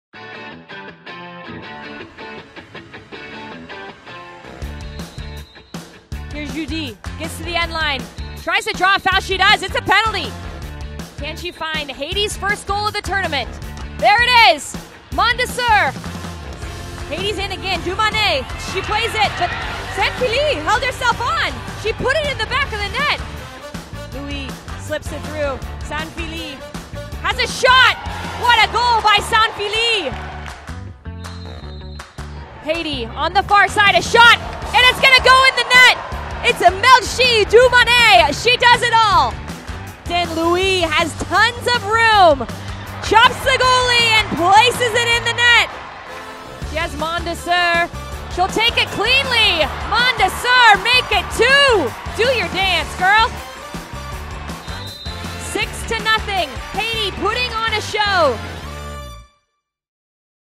Concacaf Women's Championship Qualifiers 2020: Panama vs Haiti| Highlights
📹 Haiti 🇭🇹 earned a 6-0 win over Panama 🇵🇦 today in CWOQ. 👀Don't miss the best moments!